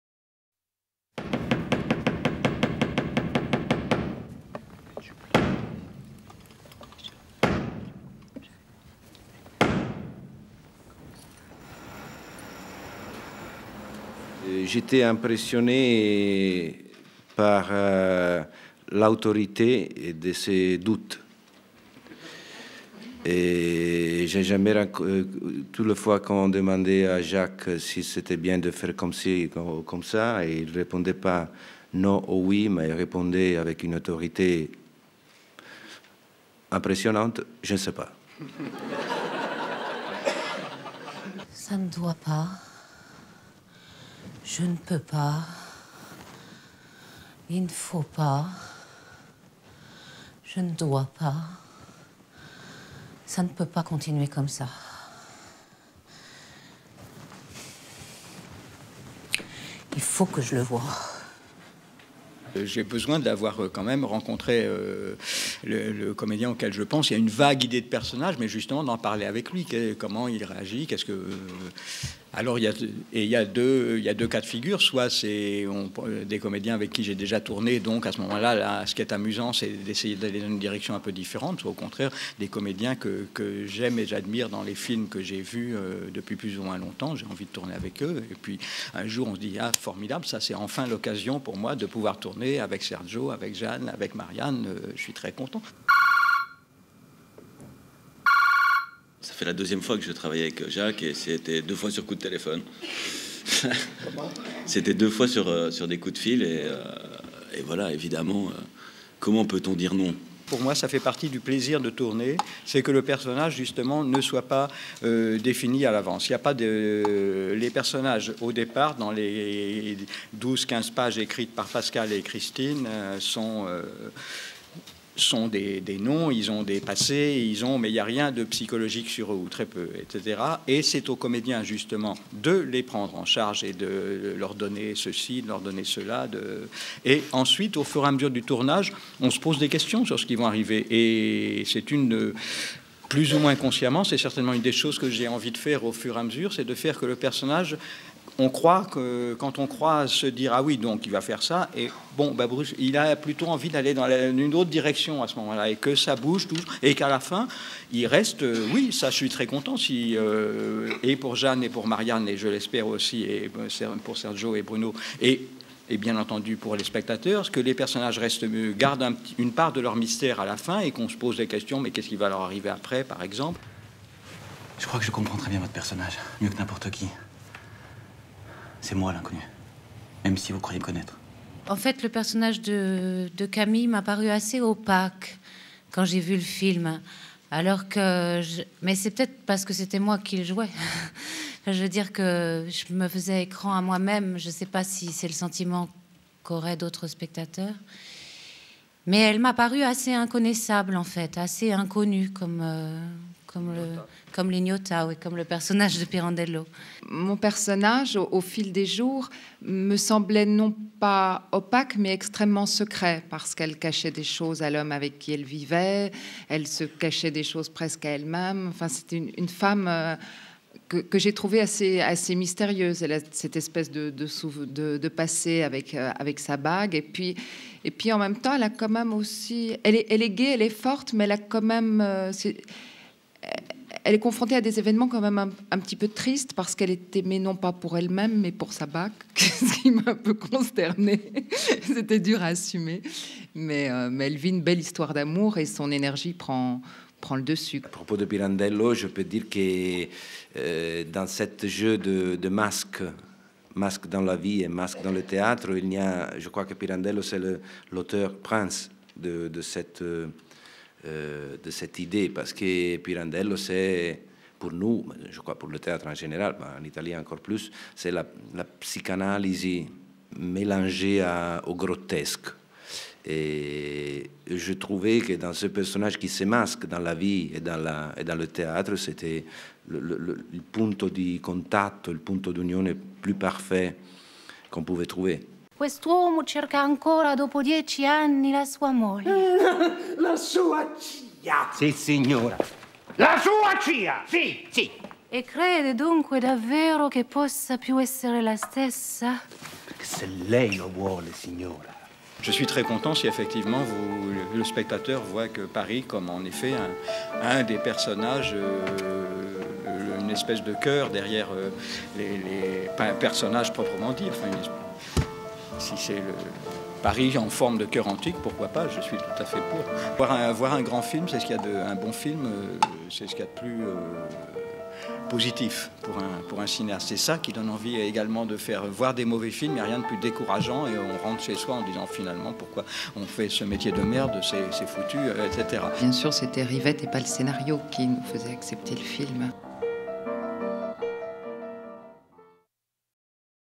Jacques Rivette interview Va Savoir 2002
Tiré des archives de DVDvision #22 ! Une Interview de Jacques Rivette à propos de son film Va Savoir réalisée par Françoise Jallot, montage par Catherine Lucet.